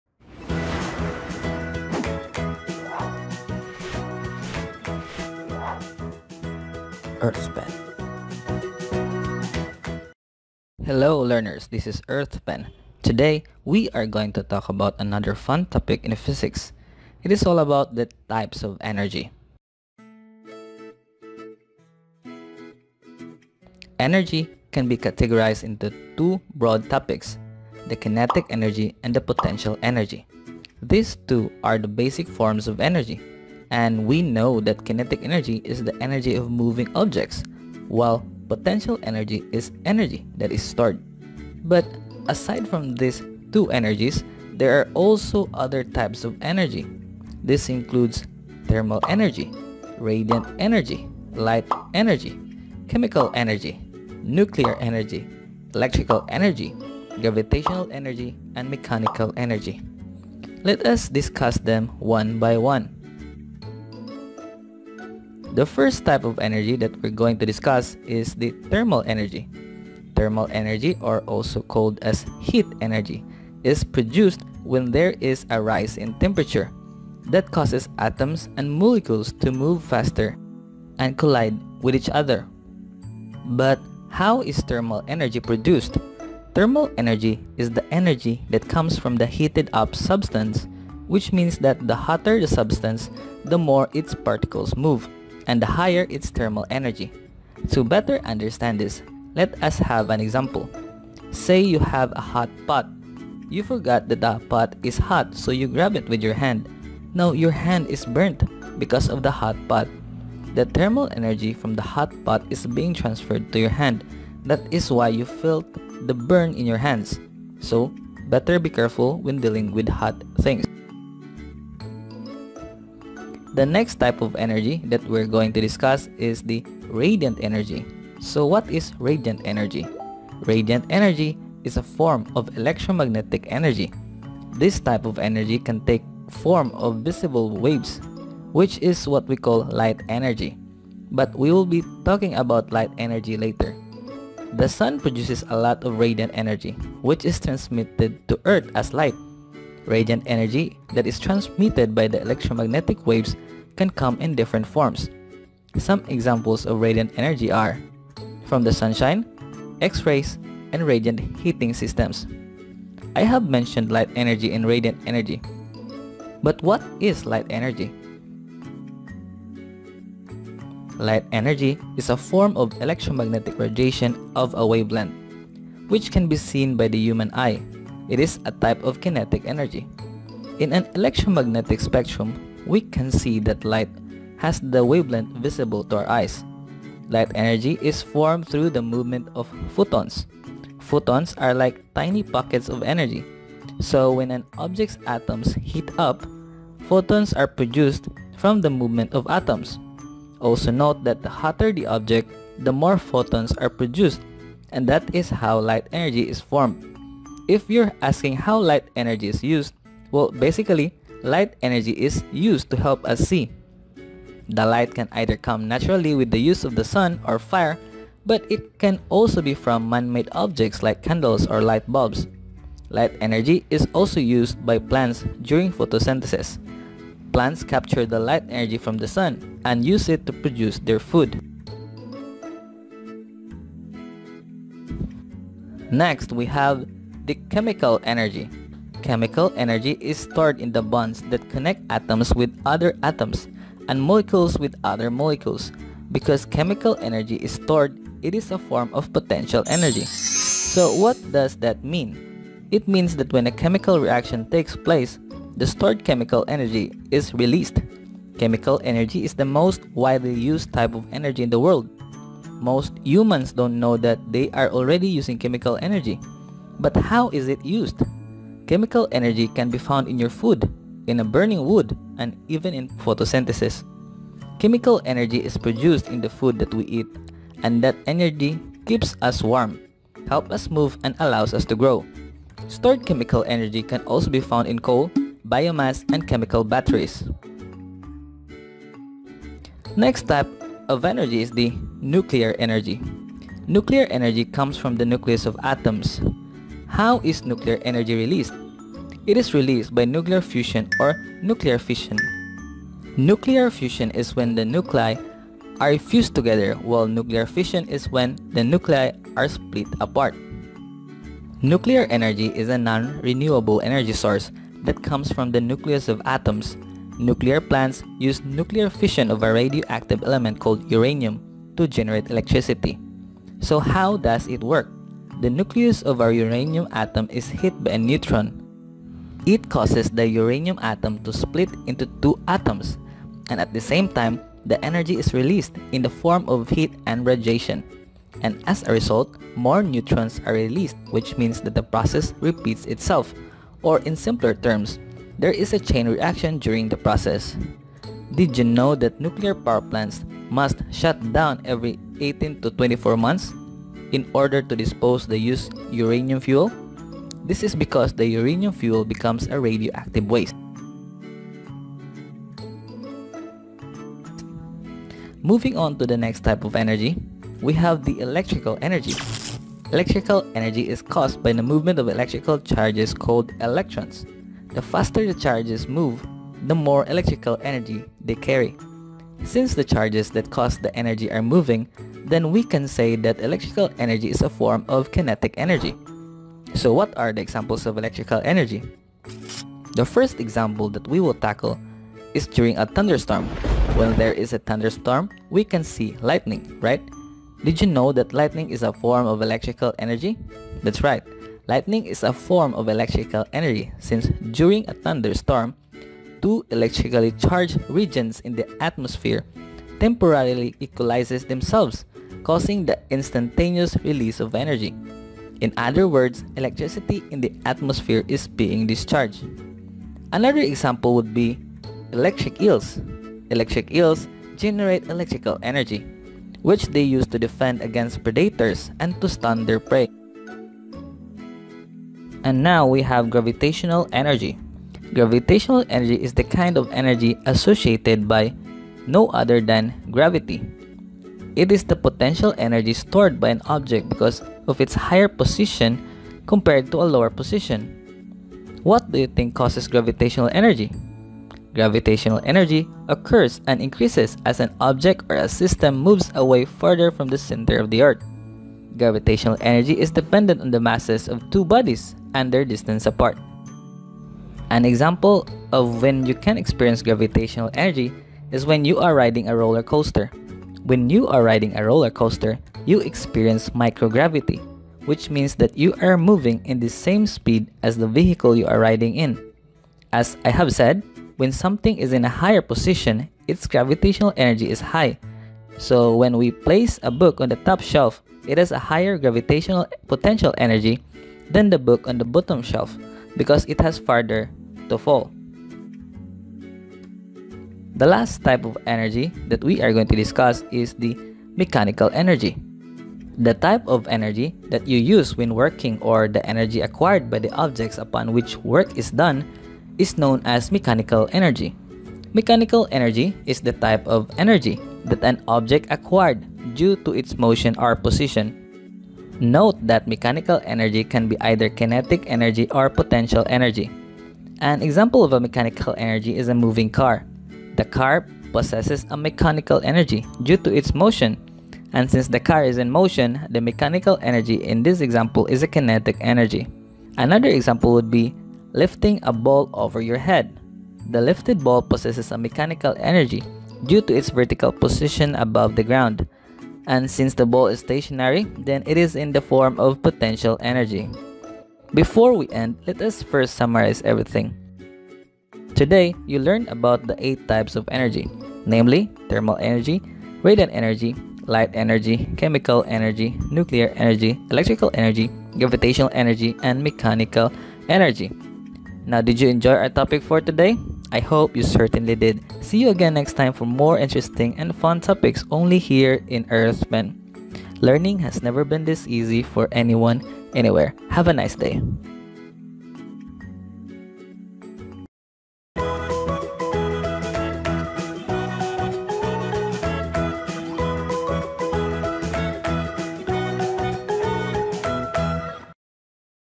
TYPES OF ENERGY | Physics Animation
Hello, Learners! This is EarthPen. Today, we are going to talk about another fun topic in Physics. It is all about the types of energy.

Energy can be categorized into two broad topics; kinetic energy and potential energy. These two are the basic forms of energy. And we know that kinetic energy is the energy of moving objects while potential energy is energy that is stored. But aside from these two energies, there are also other types of energy. This includes thermal energy, radiant energy, light energy, chemical energy, nuclear energy, electrical energy, gravitational energy, and mechanical energy.
Let us discuss them one by one.

The first type of energy that we’re going to discuss is thermal energy. Thermal energy or also called heat energy is produced when there is a rise in temperature that causes atoms and molecules to move faster and collide with each other.
But how is thermal energy produced? Thermal energy is the energy that comes from the heated up substance which means that the hotter the substance, the more its particles move, and the higher its thermal energy.

To better understand this, let us have an example. Say you have a hot pot. You forgot that the pot is hot so you grabbed it with your hand. Now, your hand is burnt because of the hot pot. The thermal energy from the hot pot is being transferred to your hand that is why you felt the burn in your hands. So, better be careful when dealing with hot things.

The next type of energy that we’re going to discuss is radiant energy.
What is radiant energy? Radiant energy is a form of electromagnetic energy. This type of energy can take the form of visible waves, which is what we call light energy. But we’ll be talking about light energy later.

The sun produces a lot of radiant energy which is transmitted to Earth as light. Radiant energy that is transmitted by electromagnetic waves can come in different forms. Some examples of radiant energy are from the sunshine, x-rays, and radiant heating systems.